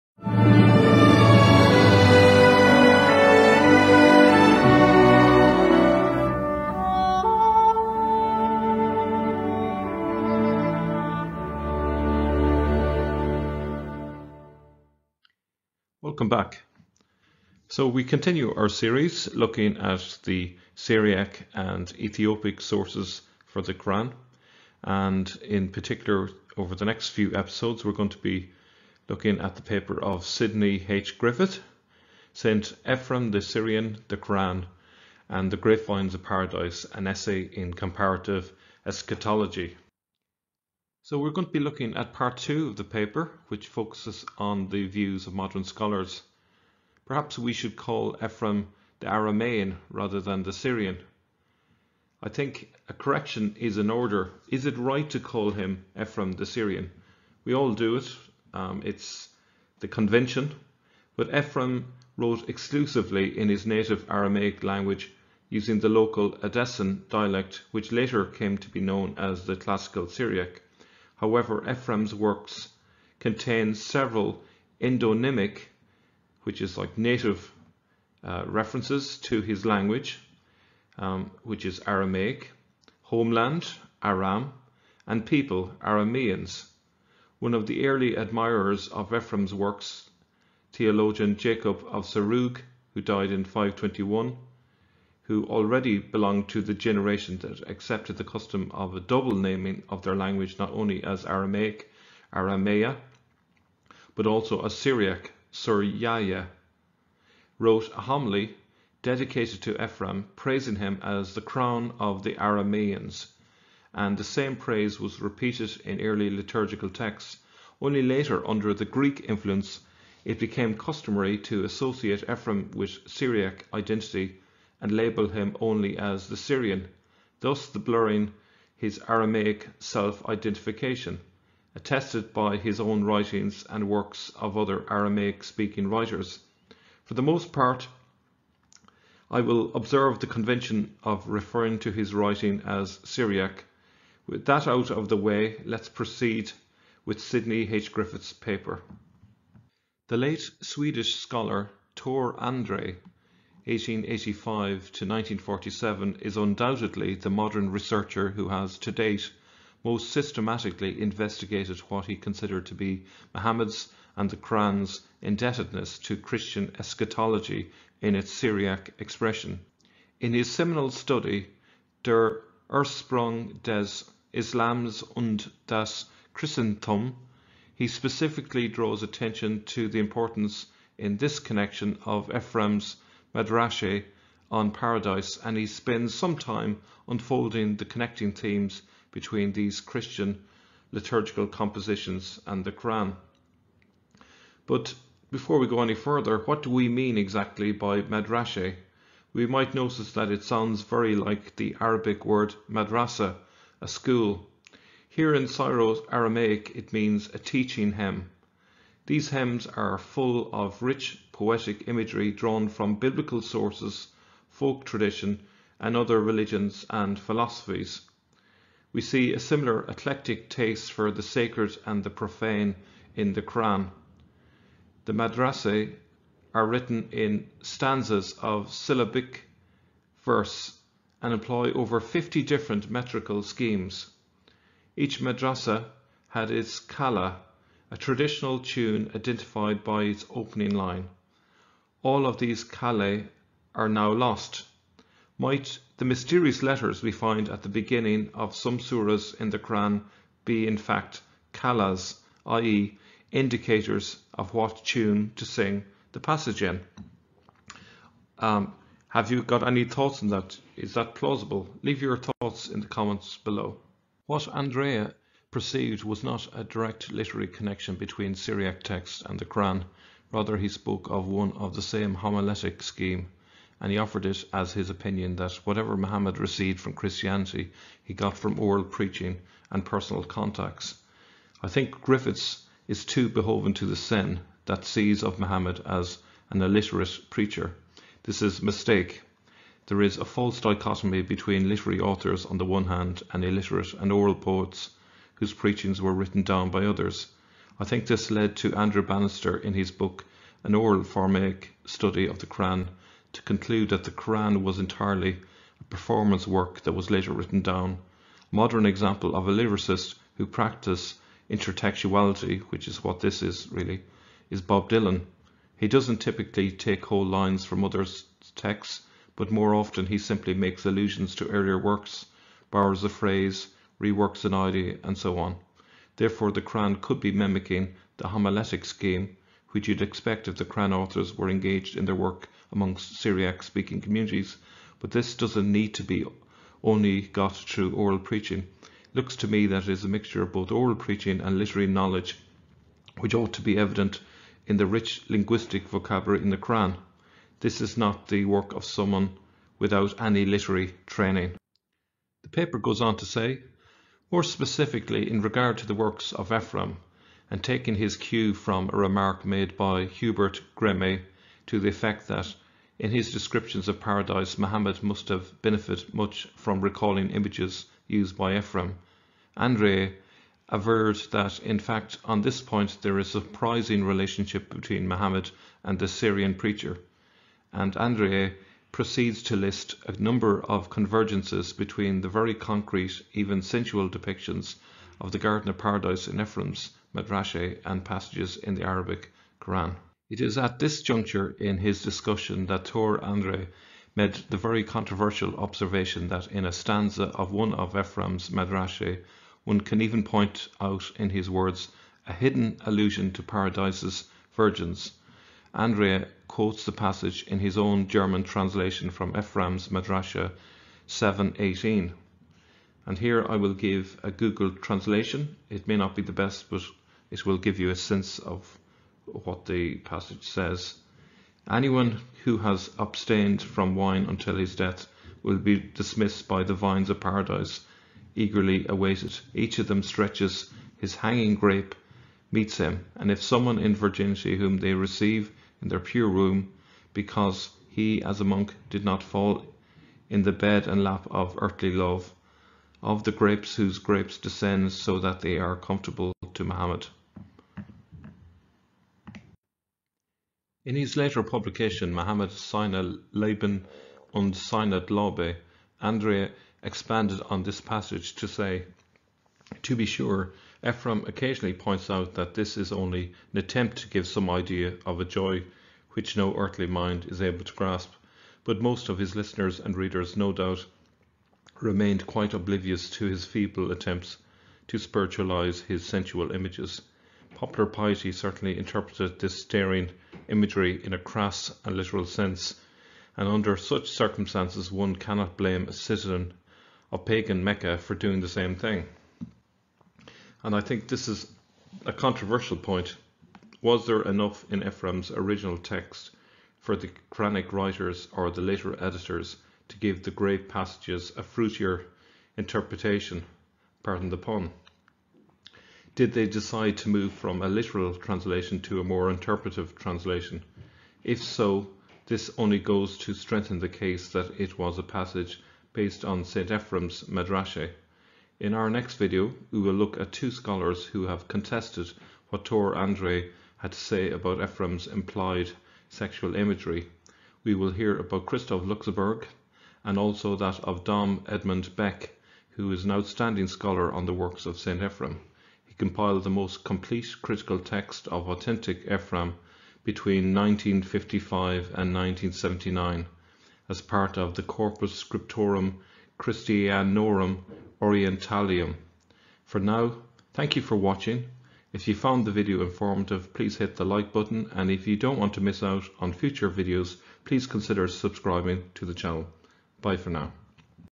# 6 Ephrem: the Syrian or the Aramaen? | Ephrem's fruity imagery [Tor Andrae] (Islamic Origins)
Mel states -
Ephrem wrote exclusively in his native Aramaic language, using the local Edessan dialect, which later came to be known as the Classical Syriac. However, Ephrem's works contain several endonymic (native) references to his language (Aramaic), homeland (Aram) and people (Arameans). One of the early admirers of Ephrem's works, theologian Jacob of Serugh (d. 521), who already belonged to the generation that accepted the custom of a double naming of their language not only as Aramaic (Ārāmāyā) but also as "Syriac" (Suryāyā), wrote a homily dedicated to Ephrem, praising him as the crown of the Arameans , and the same praise was repeated in early liturgical texts. Only later, under the Greek influence, it became customary to associate Ephrem with Syriac identity, and label him only as "the Syrian", thus blurring his Aramaic self-identification, attested by his own writings and works of other Aramaic-speaking writers.

What do we mean by Madrashe? We might notice that it sounds very like the Arabic word Madrasa, a school. Here in Syro-Aramaic, it means a “teaching hymn”. These hymns are full of rich, poetic imagery drawn from biblical sources, folk tradition, and other religions and philosophies. We see a similar eclectic taste for the sacred and the profane in the qur’an.

Was there enough in Ephrem’s original text for the quranic writers, or the later editors, to give the grape passages a fruitier interpretation? Did they decide to move from a literal translation to a more interpretative translation? If so, this only goes to strengthen the case that it was a passage based on St Ephrems’ madrashe.